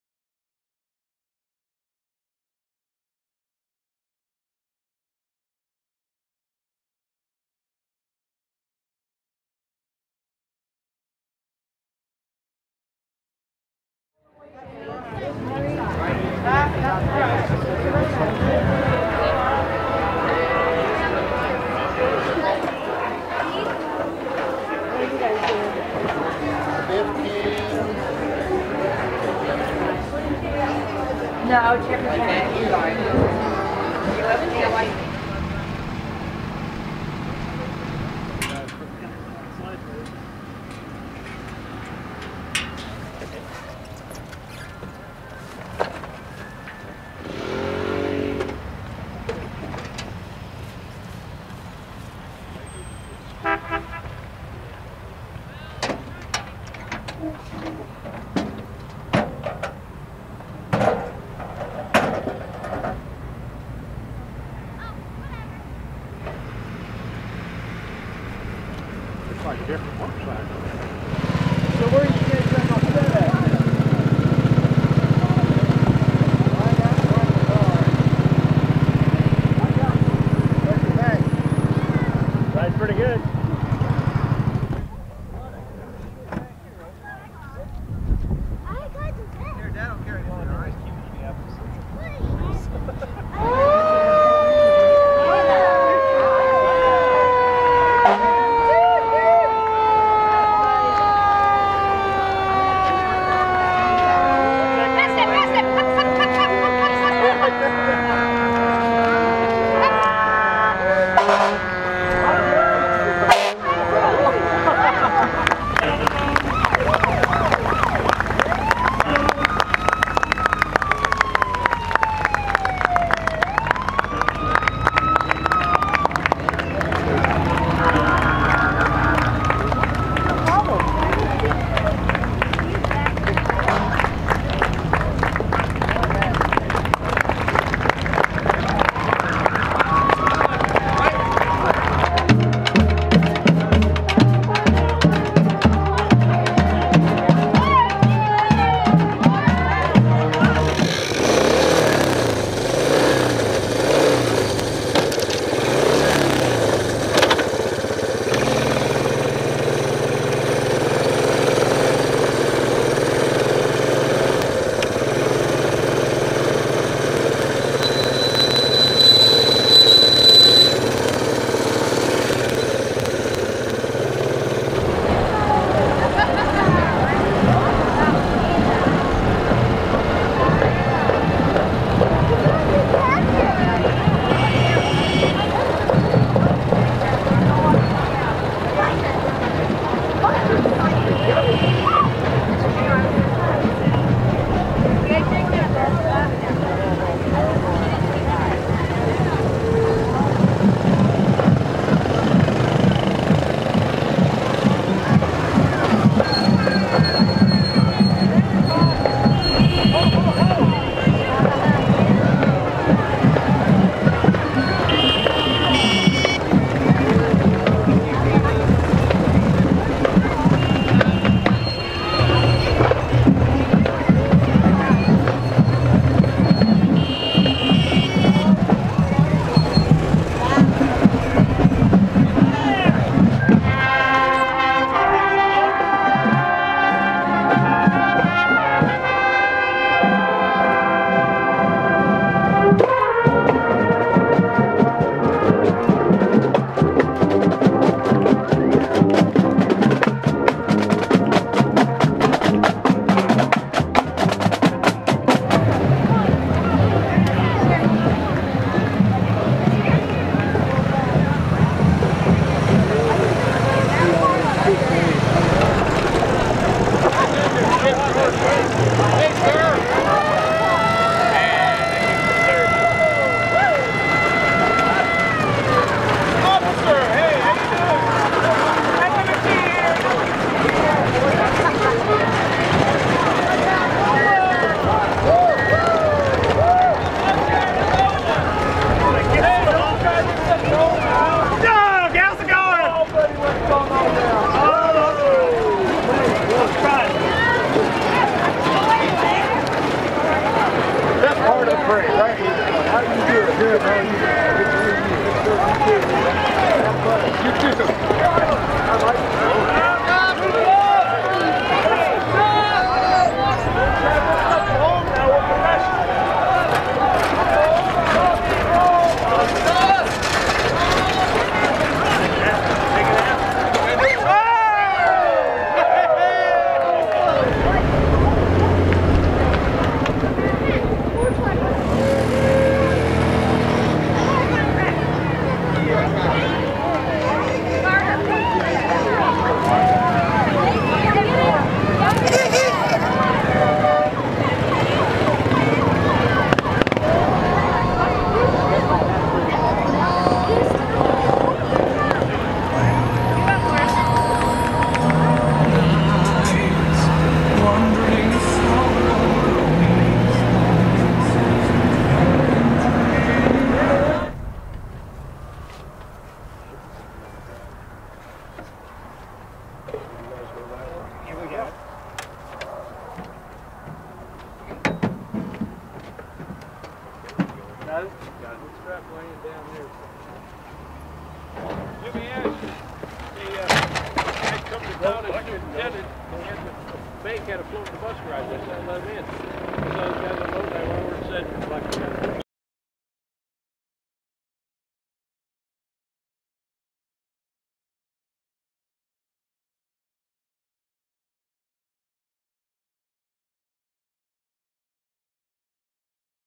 Broom Corn Festival Parade 2014
The Ansar Cycle Patrol rides in the Broom Corn Festival Parade in Arcola, Illinois, on September 6, 2014.